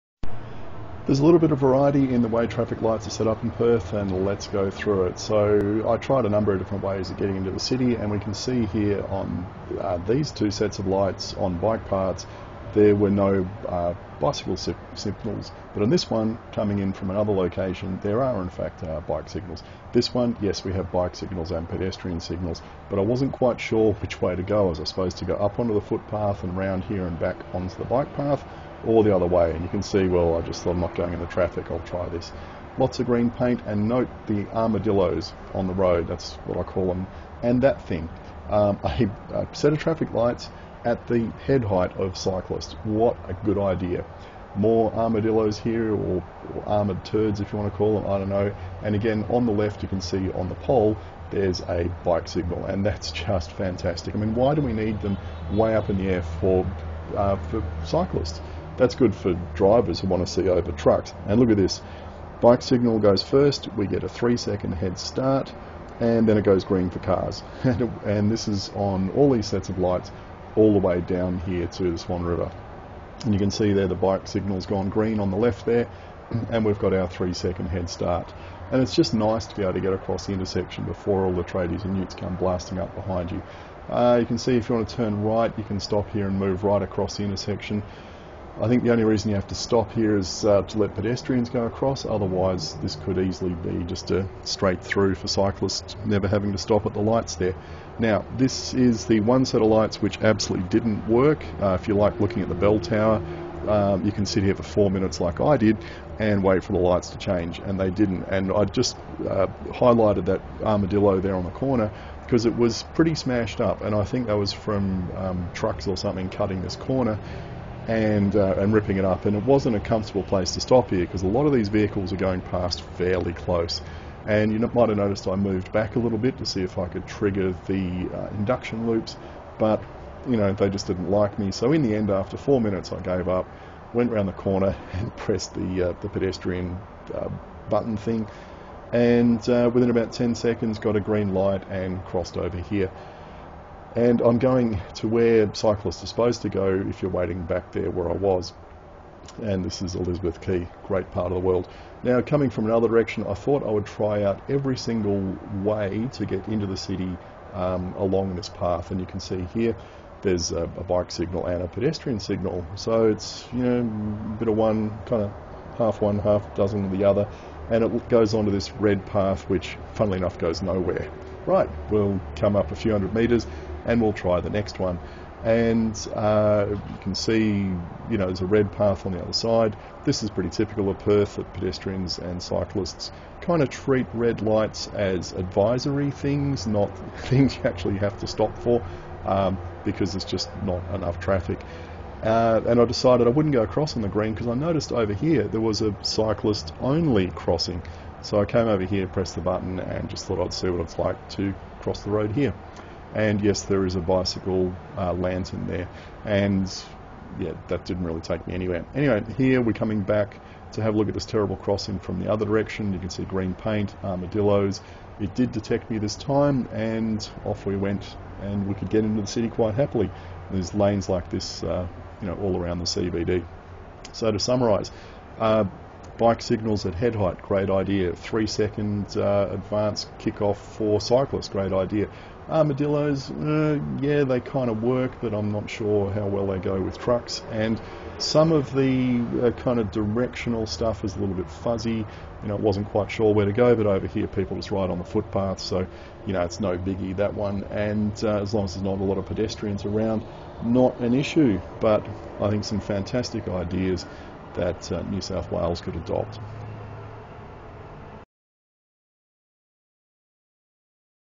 Stuff Sydney traffic signal engineers can learn from Perth
I am sure the title just triggered a bunch of TfNSW engineers and managers, but there are some very simple, but very effective solutions currently being used in the Perth CBD.
It also seems that no one cares whether a set of traffic lights has a bicycle lantern or not - it's not the end of the world if one hasn't been installed at an intersection on a bike path.